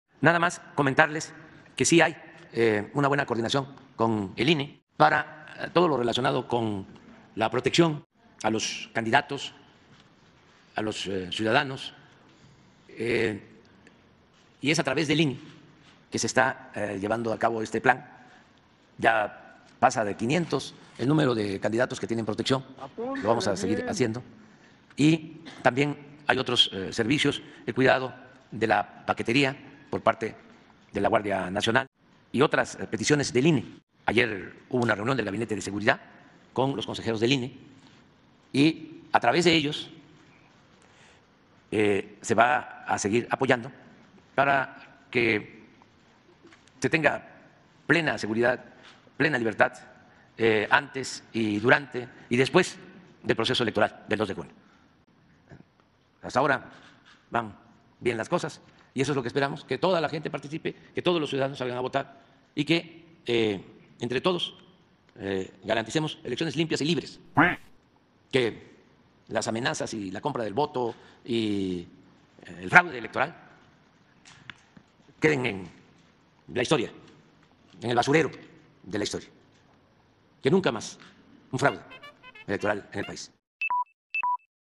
Asegura que hay buena coordinación con el INE para garantizar la protección de candidatos/as 🦆🗳️
Asegura que hay buena coordinación con el INE para garantizar la protección de candidatos/as, antes, durante y después se garantiza seguridad para que la ciudadanía salga a votar 🗳️ y que nunca vuelva haber un fraude electoral 🦆